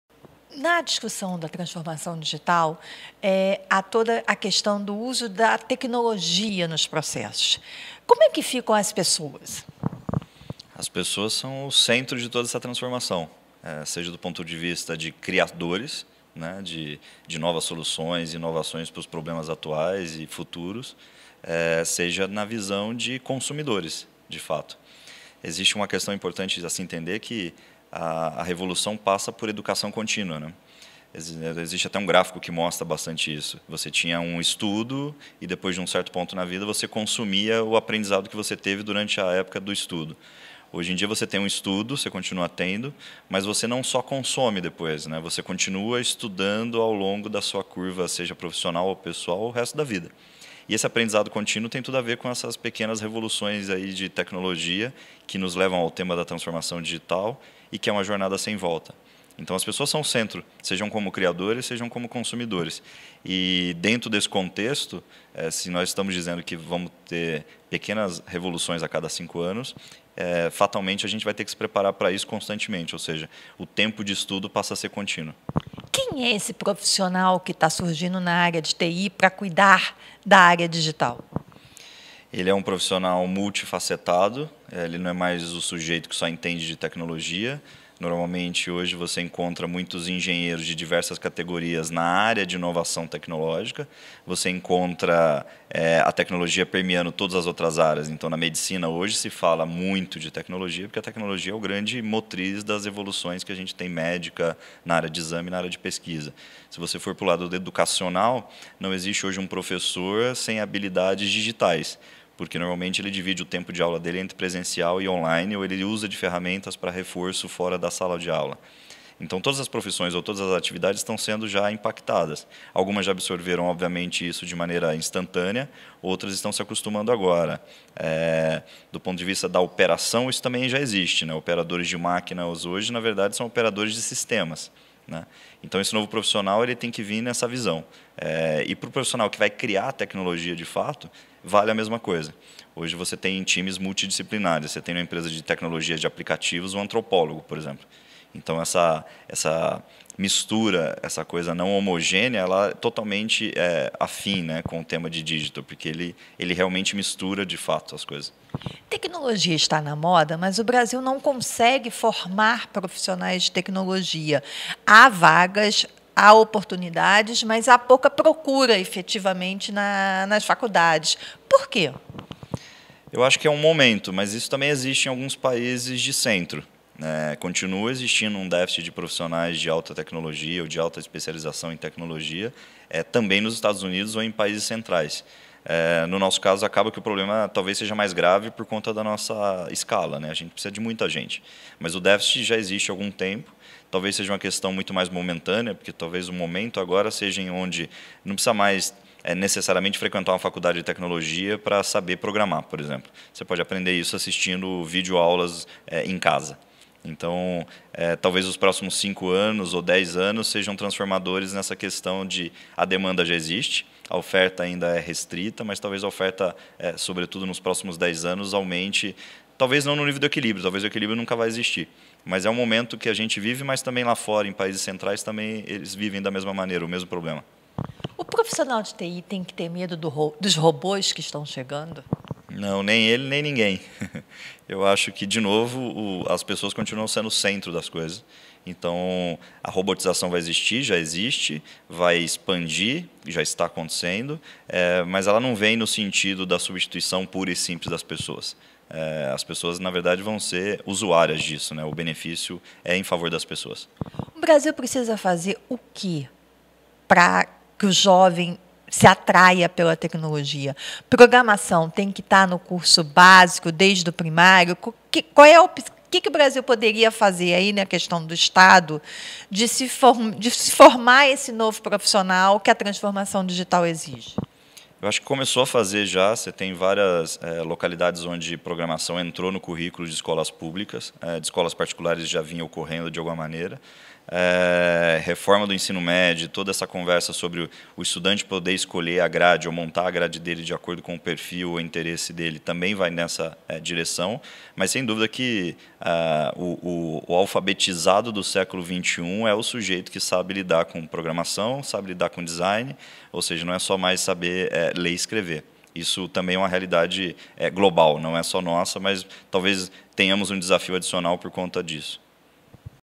Sem pessoas, empresas não alcançam a transformação digital
Os principais ativos das empresas que buscam a transformação digital são as pessoas – e não há como ter sucesso na empreitada sem profissionais de qualidade. É o que aponta o diretor executivo de mobilidade, fluig & analytics da Totvs, Mario Almeida, ao colocar as pessoas como pontapé inicial das mudanças.

“As pessoas são o centro de toda essa transformação, seja do ponto de vista de criadores de novas soluções e inovações, seja na visão de consumidores. A revolução passa por educação contínua. Você tinha um estudo e durante certo ponto da vida consumia esse aprendizado. Hoje em dia você tem estudo, mas não só consome depois, continua estudando ao longo da curva profissional ou pessoal o resto da vida”, disse o executivo ao participar do 2º Seminário Brasscom Políticas Públicas & Negócios, realizado nos dias 15 e 16 de março, em Brasília.

Esse profissional, porém, tem que estar preparado para a nova realidade. “É um profissional multifacetado. Não é mais o sujeito que só entende de tecnologia. Hoje você encontra muitos engenheiros de diversas categorias na área de inovação tecnológica. A tecnologia permeia todas as áreas. Então na medicina hoje se fala muito de tecnologia. Se for para o lado educacional, não existe hoje professor sem habilidades digitais. Todas as atividades estão sendo impactadas. Algumas já absorveram isso, outras estão se acostumando.”

Para o executivo da Totvs, ferramentas hoje disponíveis podem ajudar, ainda que não resolvam completamente, a demanda pelo pessoal qualificado que aponta como essencial. “Isso também existe em países de centro. Continua havendo déficit de profissionais de tecnologia nos Estados Unidos e países centrais. No nosso caso o problema talvez seja mais grave por conta da nossa escala, a gente precisa de muita gente. Talvez seja uma questão momentânea, porque hoje não se precisa mais necessariamente frequentar uma faculdade de tecnologia para saber programar. Dá para aprender assistindo videoaulas em casa.” Assistam a entrevista.